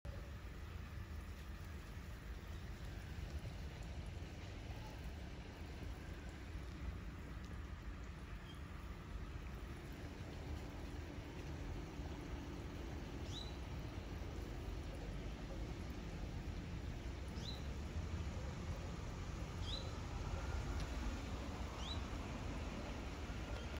Tholymis tillarga (dimicant) et Orthetrum sabina s.
Tholymis tillarga (Fabricius, 1798) dimicant, et
Orthetrum sabina sabina (Drury, 1770)

アメイロトンボ 繩張り爭ひ、
ハラボソトンボ